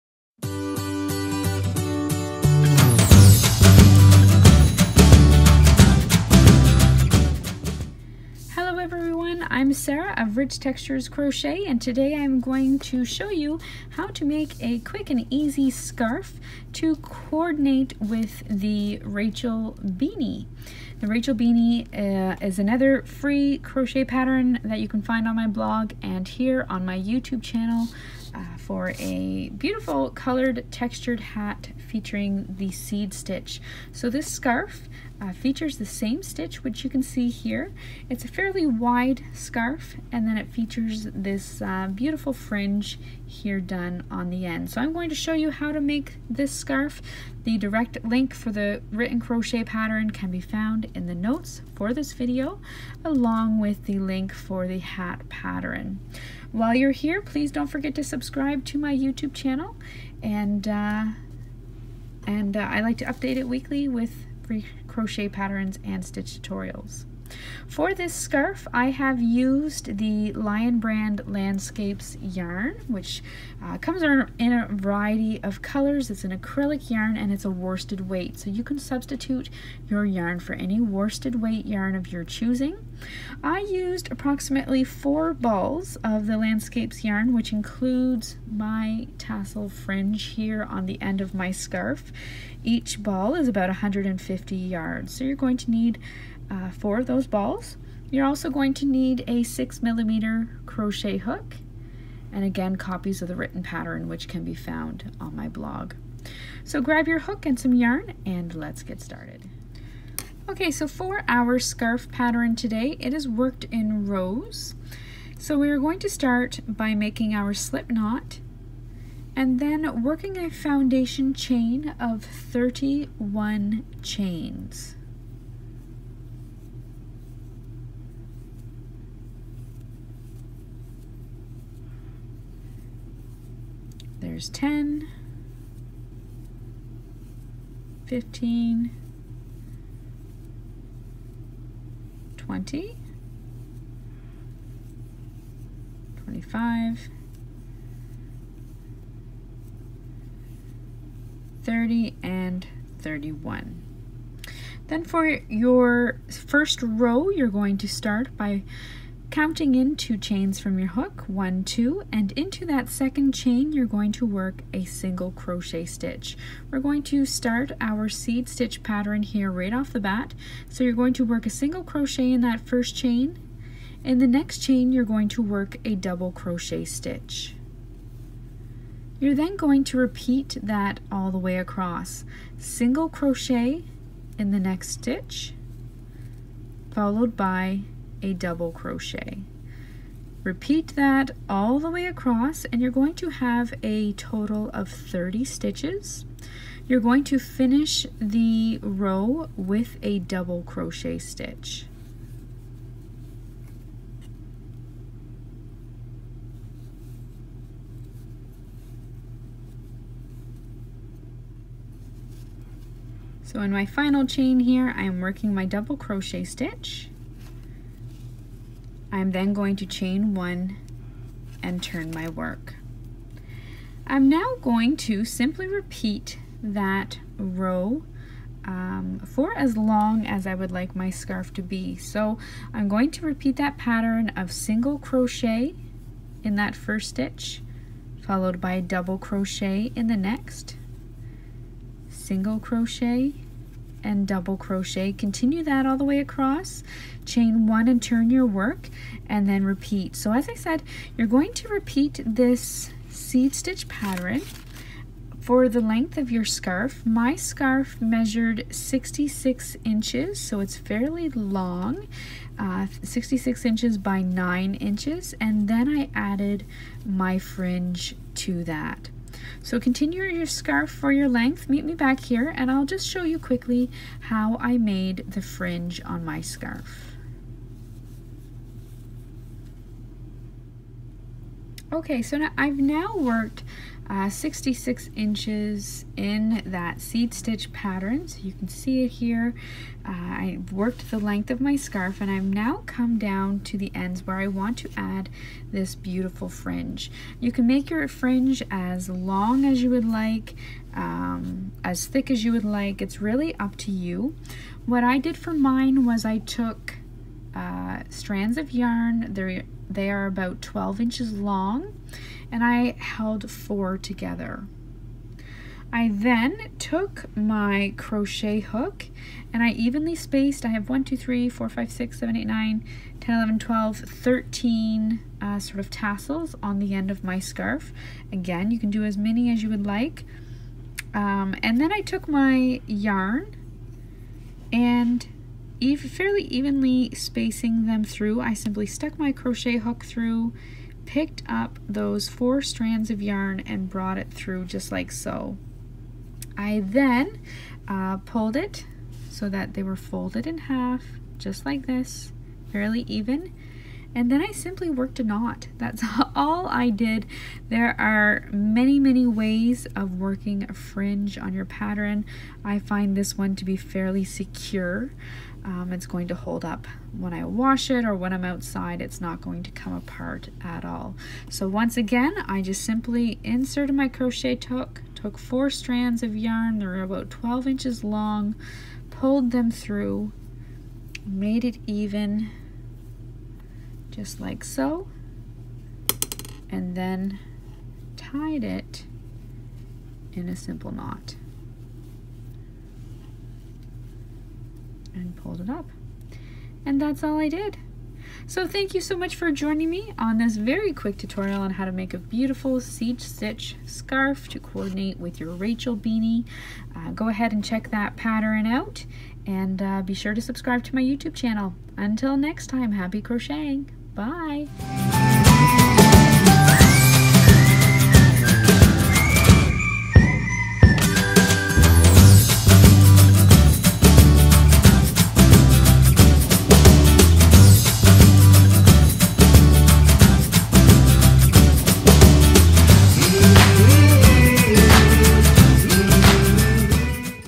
Rachel Scarf - Easy Crochet Scarf
Today we are going to learn how to crochet the Rachel scarf! This scarf complements the Rachel Beanie it is made using the beautiful seed stitch and made using Landscapes Yarn by Lion Brand Yarn.

Gauge: 12 sts x 11 rows of seed stitch = 4 inches

Finished Size:   Scarf: 9 x 66 inches (Excluding Fringe)

Yarn: Landscapes® by Lion Brand® (100% Acrylic; 5.3 oz, 150 g; Approx. 147 yds, 134 m). Worsted/medium Weight (4). You will need 4 balls for the scarf.

Hook: Size J/10 (6 mm) Hook. Adjust hook size if necessary to obtain correct gauge.

Sarah